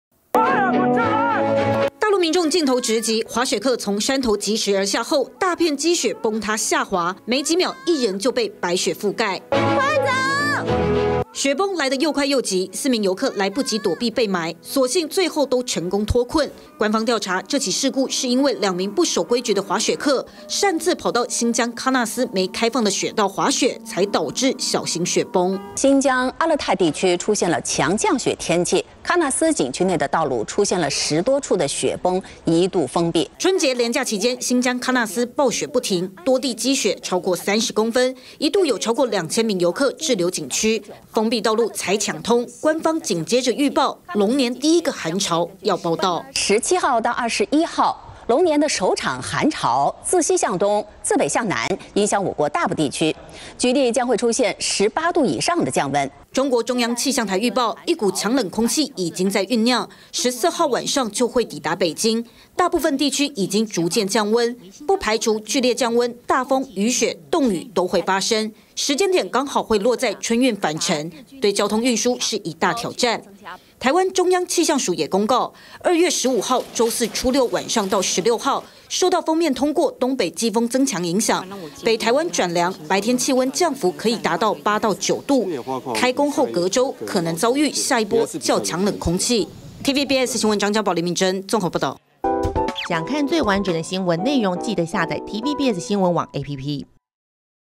龍年首波寒流2/17報到 陸局部驟降18度｜TVBS新聞
這次春節假期，大陸、台灣基本上都是溫暖好天氣。但準備開工也要準備好厚一點的衣服了。初六受到東北季風增強影響，北部白天有機會降溫9度。大陸氣象局也預報，龍年第一個寒潮可能在17號報到，部分地區降溫會超過18度。